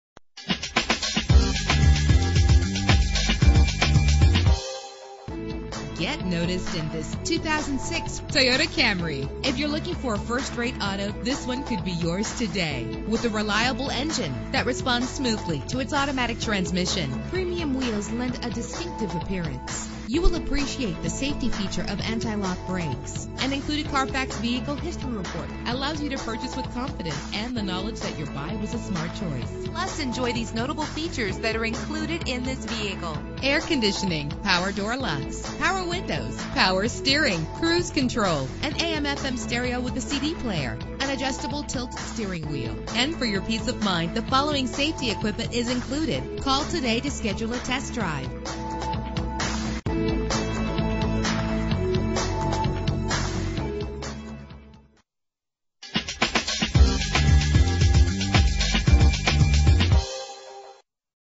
2006 Toyota Camry - Pensacola FL
Year: 2006
Make: Toyota
Model: Camry
Trim: LE Sedan 4D
Engine: 4 cylinder 2.4 Liter
Transmission:
Color: Silver
Mileage: 49504
Address:
7201 Pensacola Blvd